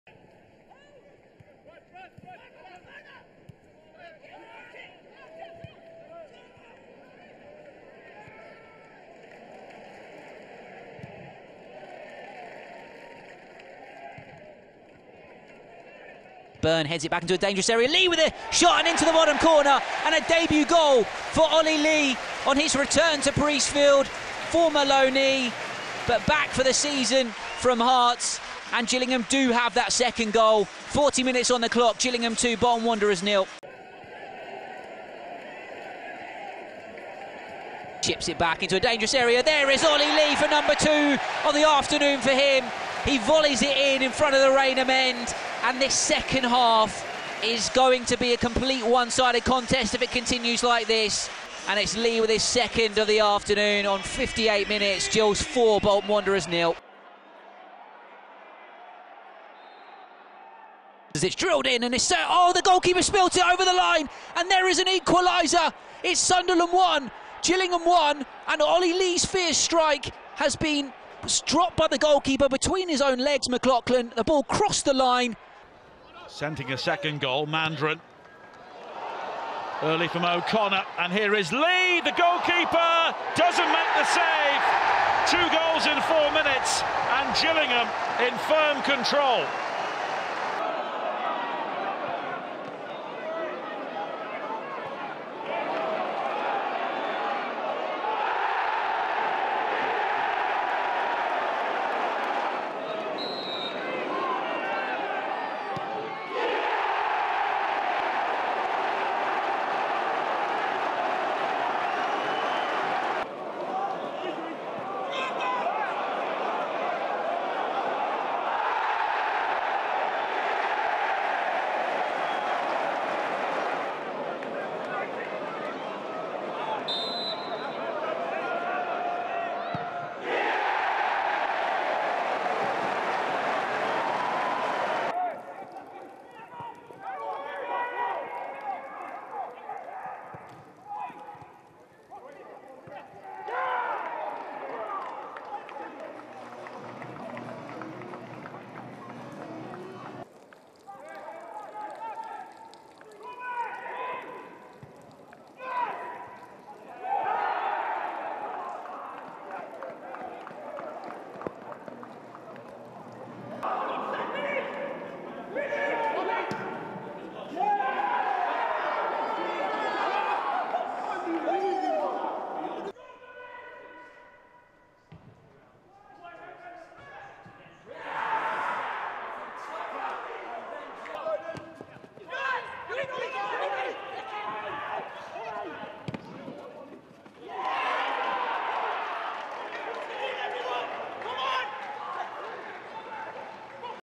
OLLY LEE'S GILLS GOALS (2019/20 & 2020/21)
Olly Lee has rejoined the Gills on a permanent basis following three previous loan spells at Priestfield.

Check out all 11 of his goals scored so far, in 66 appearances.